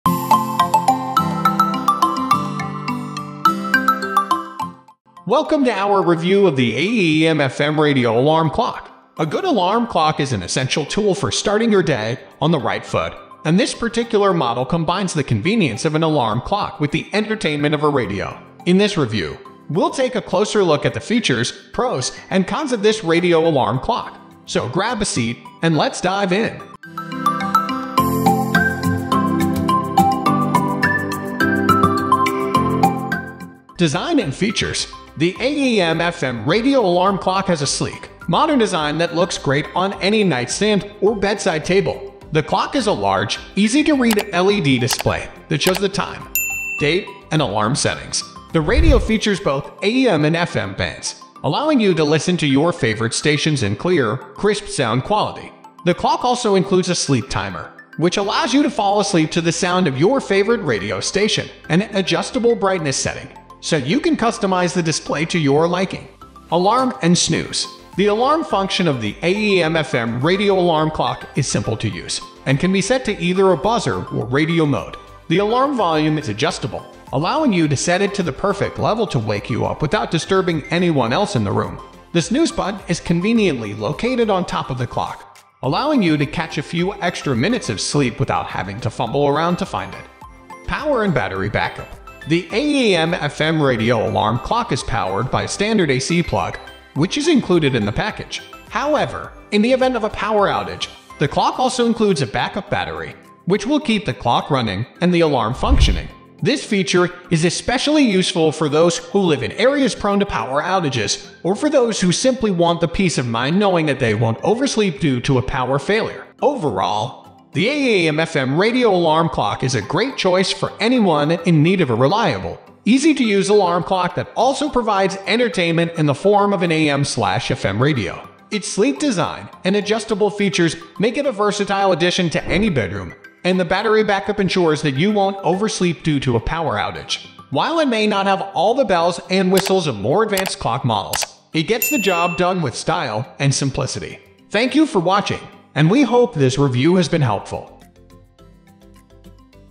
AM FM Radio Alarm Clock Review | Wake Up to Your Favorite Tunes!
Welcome to our comprehensive review of the AM FM Radio Alarm Clock! In this video, we dive deep into the features, functionality, and performance of this versatile alarm clock that combines classic radio capabilities with a modern design. Whether you're a music lover or simply looking for a reliable alarm clock to wake you up, this review has got you covered.

We start by exploring the various tuning options, allowing you to easily switch between AM and FM radio stations and find your favorite frequencies. Discover the crystal-clear sound quality and the convenience of preset channels for quick access to your preferred stations.

Next, we examine the alarm settings, providing a step-by-step guide on how to set up your wake-up calls. Say goodbye to harsh beeping noises in the morning and wake up to your favorite radio station or a soothing alarm tone. We discuss the different alarm modes, snooze options, and how to customize your wake-up experience.

Furthermore, we delve into the clock's design and build quality, highlighting its sleek appearance, user-friendly interface, and durable construction. We explore the display features, including adjustable brightness levels and easy-to-read digits, ensuring you can check the time with a quick glance.

To enhance your user experience, we discuss additional features such as sleep timers, backup battery power, and the convenience of a built-in USB charging port for your devices. We also touch on the compact size, making it an ideal travel companion for those on the go.

If you're interested in purchasing an AM FM Radio Alarm Clock, this review provides valuable insights to help you make an informed decision. Join us as we showcase the pros and cons of this product, empowering you to select the perfect alarm clock that suits your needs and preferences.